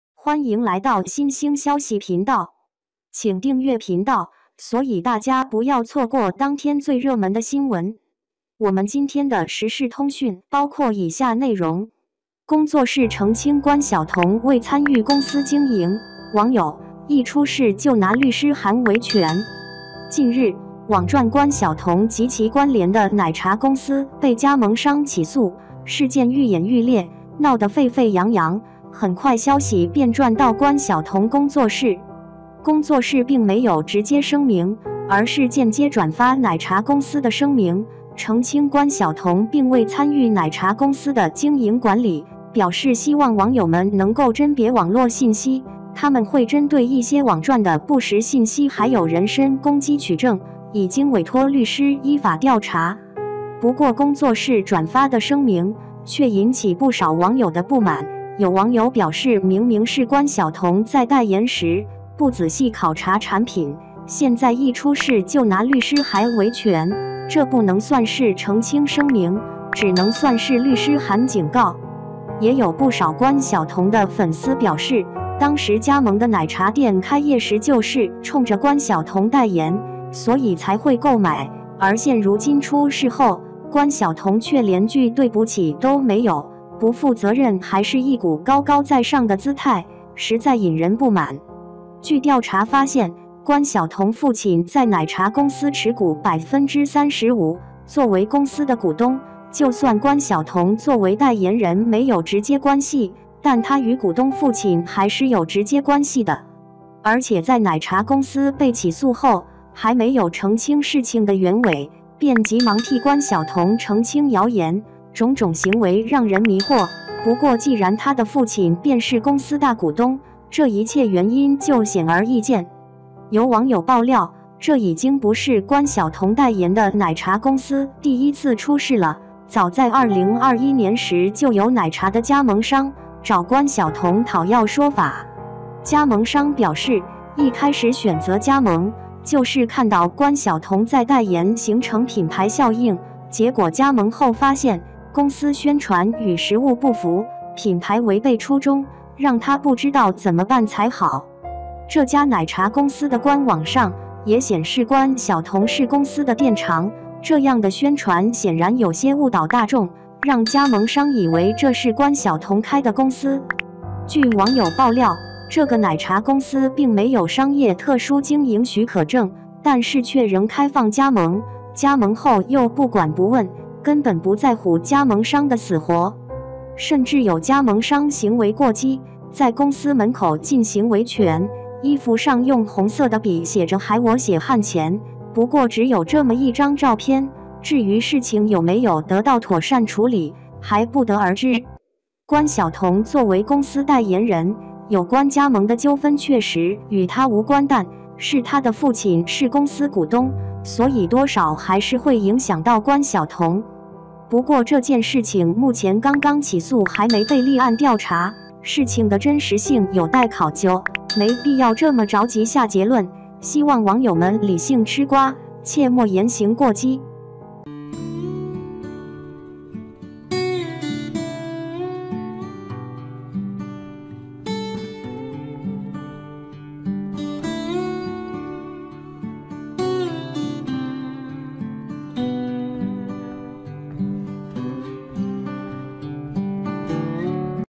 工作室澄清关晓彤未参与公司经营，网友：一出事就拿律师函维权
工作室澄清关晓彤未参与公司经营，网友：一出事就拿律师函维权 | 新星消息